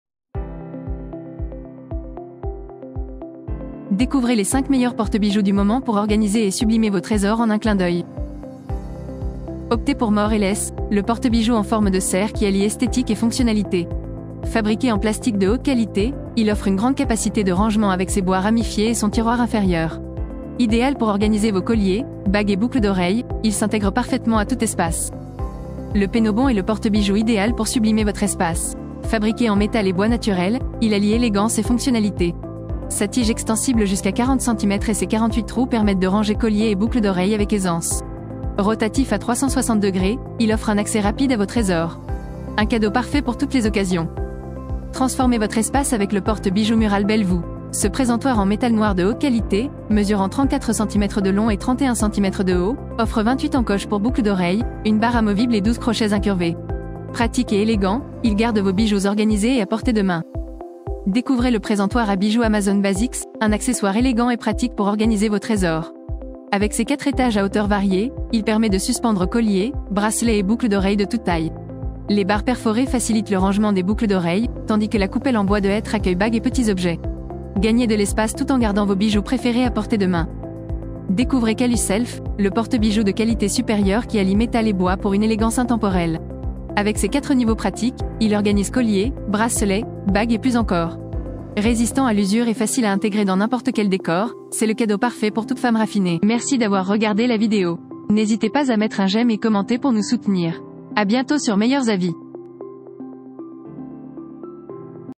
⭐️ MEILLEUR PORTE BIJOUX - Comparatif 2024
⭐️ Porte bijoux Caluself
⭐️ Porte bijoux Amazon Basics
⭐️ Porte bijoux Belle Vous
⭐️ Porte bijoux Penobon
⭐️ Porte bijoux More & less